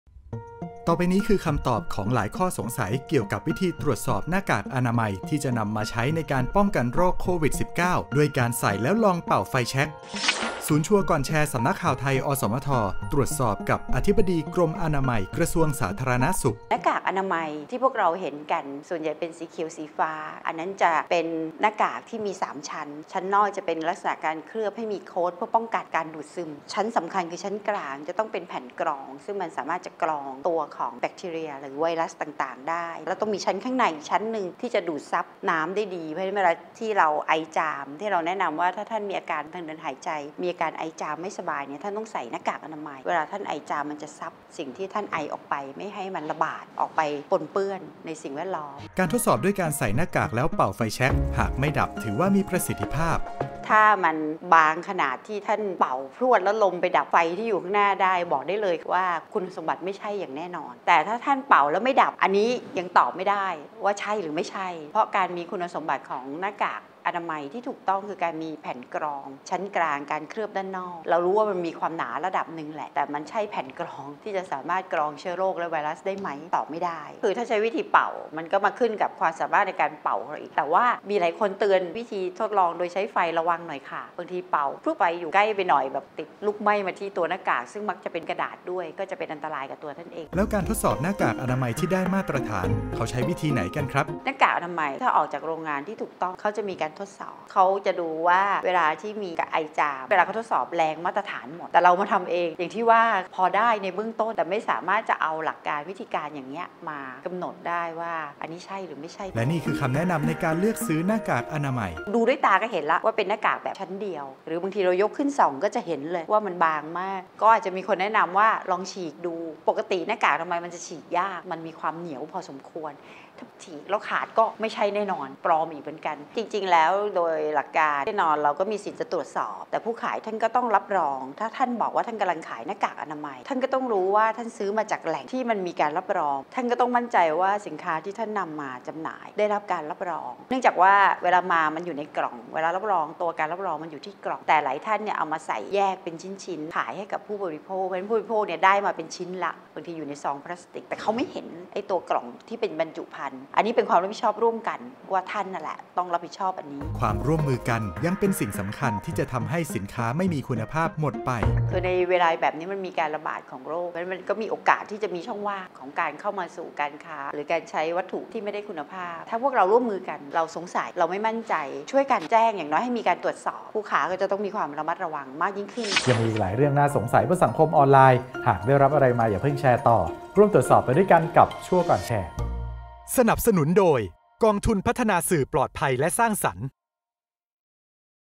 วิธีตรวจสอบหน้ากากอนามัยด้วยการเป่าไฟแช็ก จริงหรือ ? | ความจริง-โควิด | ชัวร์ก่อนแชร์ FACTSHEET
...บนสังคมออนไลน์มีหลายข้อสงสัยเก่ียวกับวิธีการตรวจสอบหน้ากากอนามัยด้วยการใส่ แล้วลองเป่าไฟแช็ก หากเป่าแล้วไม่ดับ ถือว่ามีประสิทธิภาพ จริงหรือ ?

ข้อเท็จจริงเป็นอย่างไร ร่วมตรวจสอบไปกับคุณพีรพล อนุตรโสตถิ์ จากศูนย์ชัวร์ก่อนแชร์ และ แพทย์หญิงพรรณพิมล วิปุลากร วิทยากรจาก อธิบดีกรมอนามัย กระทรวงสาธารณสุข

ตรวจสอบความจริง โดย พงศ์อิทธิ์ เชิดชูวงศ์

สรุป : วิธีที่ปรากฎในคลิปไม่ใช่ ❌ วิธีการทดสอบหน้ากากอนามัย ที่ใช้ทดสอบในทางการแพทย์ เนื่องจากแรงในการเป่าลมของแต่ละคนไม่เท่ากัน และอาจจะต้องใช้ความระมัดระวังเนื่องจากไฟอาจจะลุกไหม้หน้ากากอนามัยได้

ส่วนการการทดสอบหน้ากากอนามัยที่ถูกต้องจะแรงดันในการทดสอบ ดังนั้น คำแนะนำในการเลือกซื้อหน้ากากอนามัยจะต้องตรวจสอบจากแหล่งที่มาที่น่าเชื่อถือได้

พบกันทุกวันใน ข่าวค่ำ ช่อง 9 MCOT HD